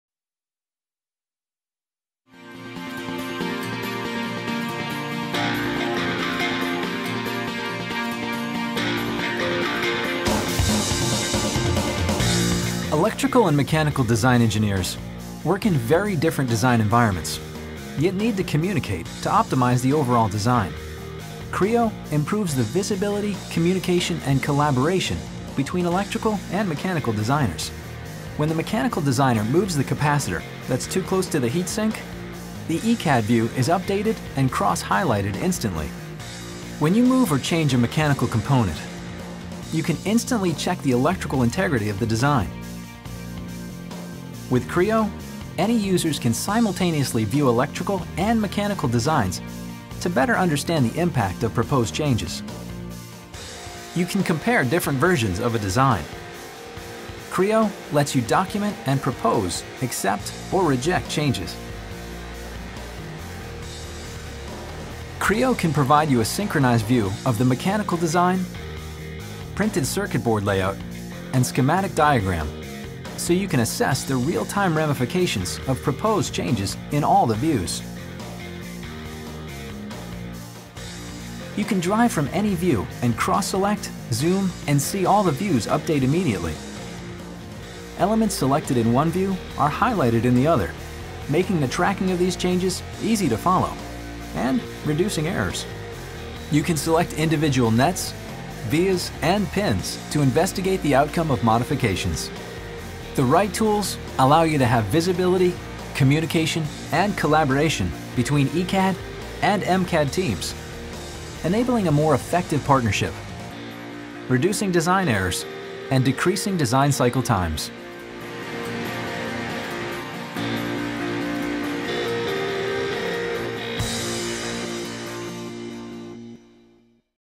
Electromechanical Codesign - PTC
Delivering innovative electromechanical designs is challenging for even the most experienced teams. Communicating design changes between disparate electrical and mechanical design solutions can be cumbersome and inefficient. Coordinating design efforts between functionally or geographically dispersed teams adds even more complexity to the challenge. Creo improves the visibility, communication and collaboration between electrical and mechanical designers.